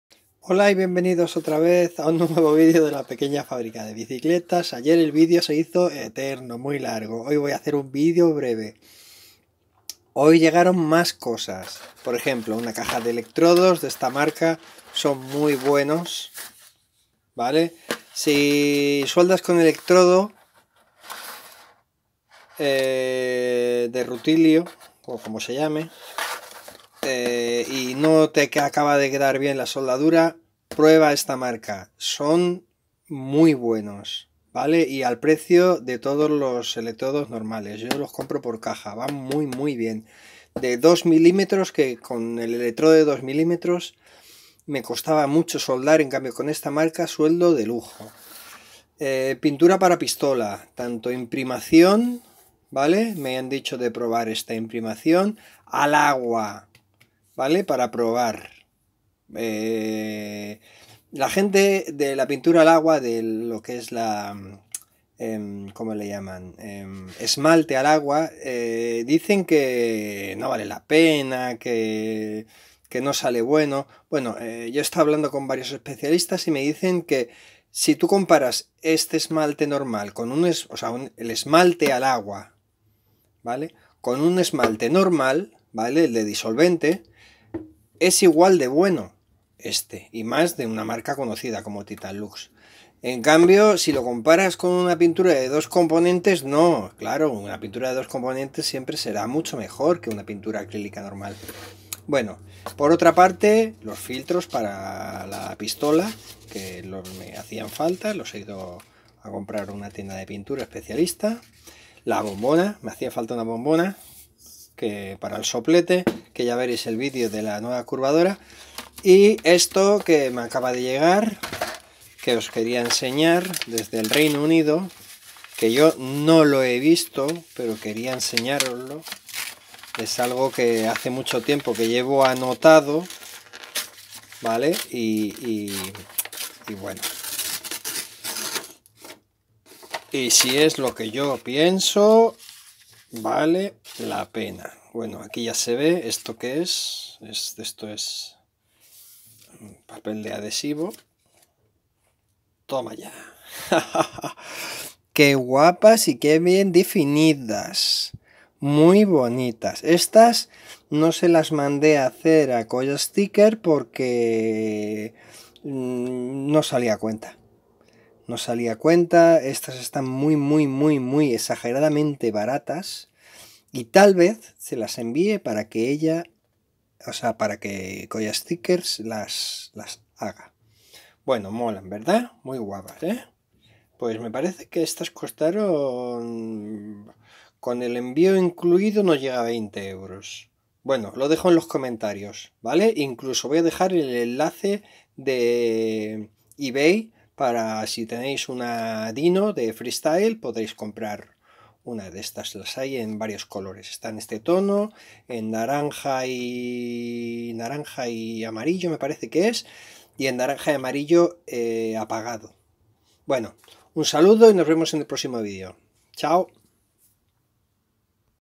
Compra de la semana 2° parte. Restauración bmx old school
El vídeo de ayer se hizo muy largo y por eso hoy vamos a hacer uno igual pero más corto también porque veo que os ha gustado. Hoy lo que vemos en el vídeo es otras compras que quizás os interese si tenéis un taller o si os vais iniciar a la pintura a pistola y por último unos adhesivos vinilos que hace mucho tiempo llevo queriendo comprar.

Enlace de pegatinas vinilos dyno compe bmx: